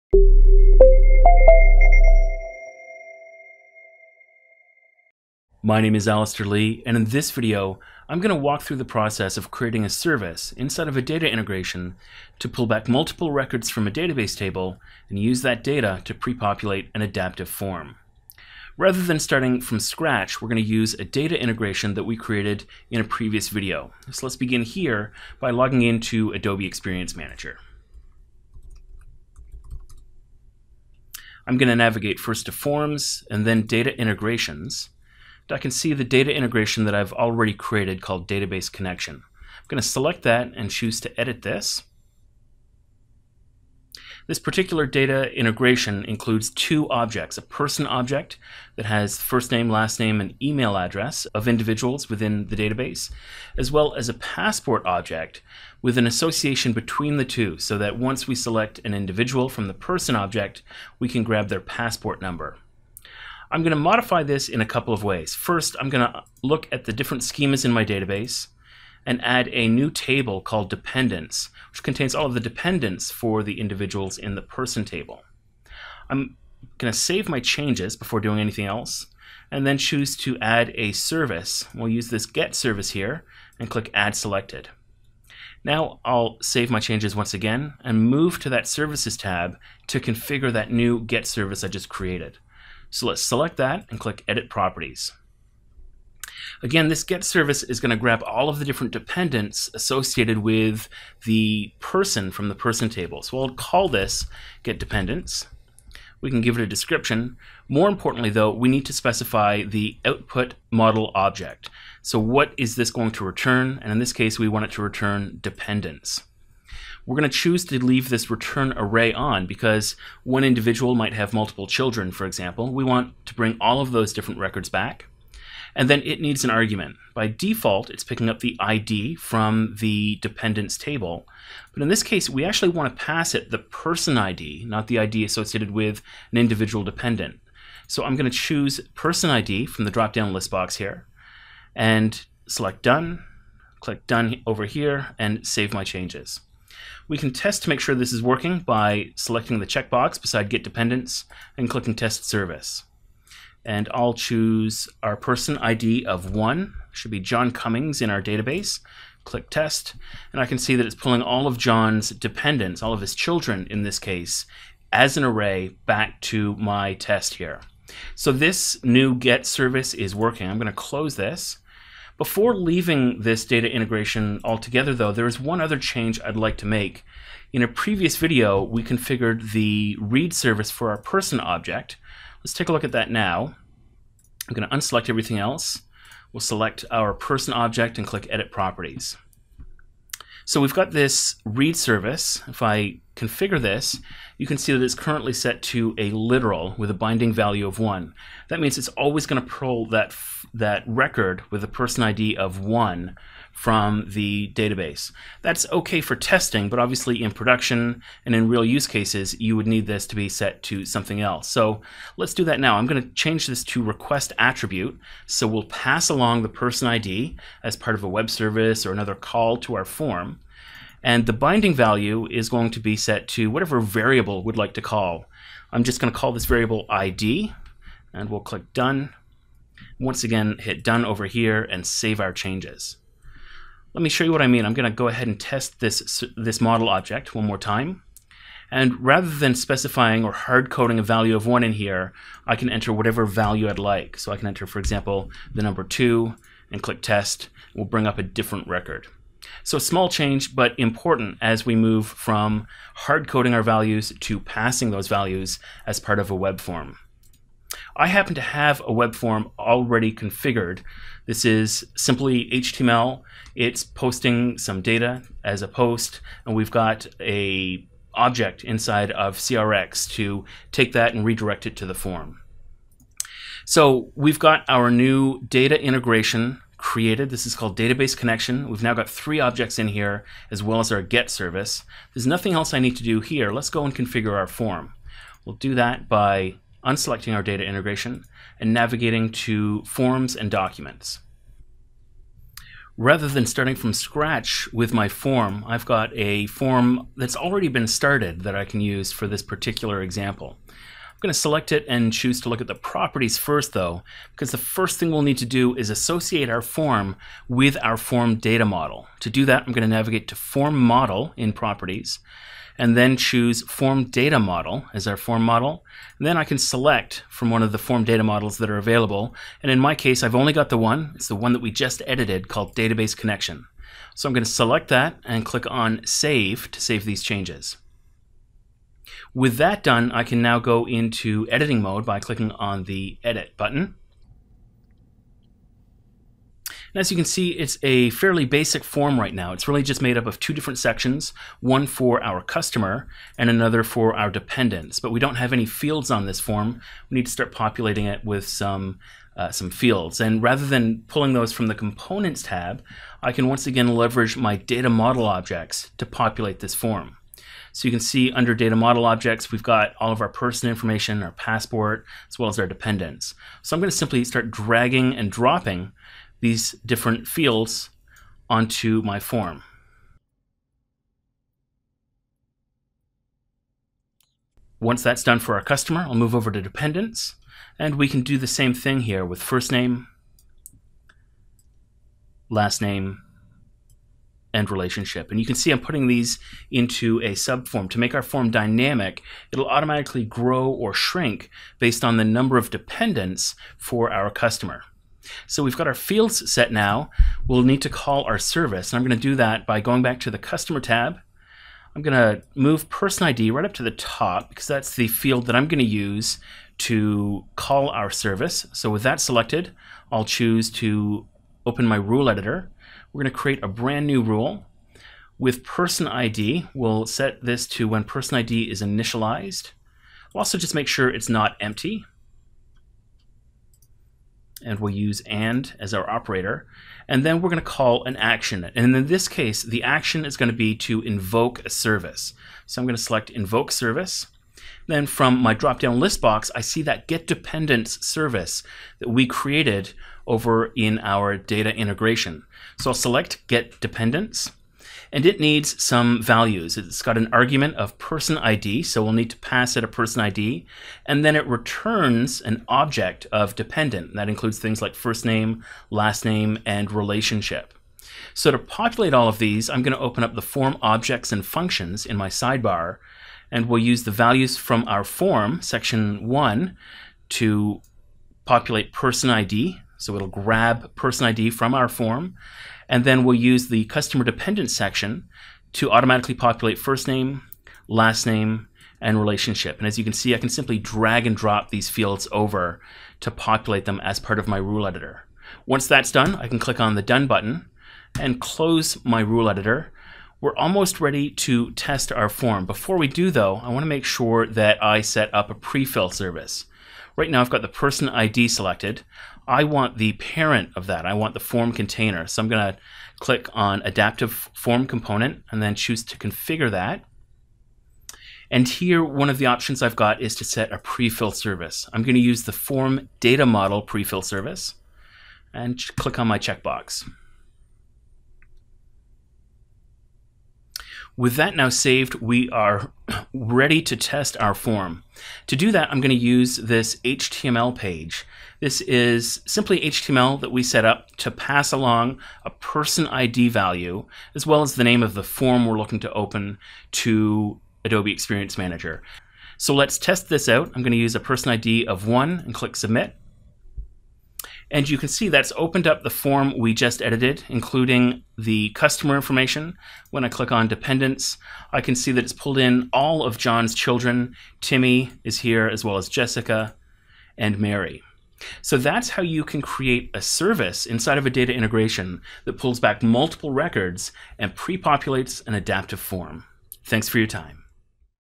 Creating a Service within a Data Integration
This video illustrates how to configure a service to work with a database. In this example, we use a GET service to lookup customer data based on a customer ID.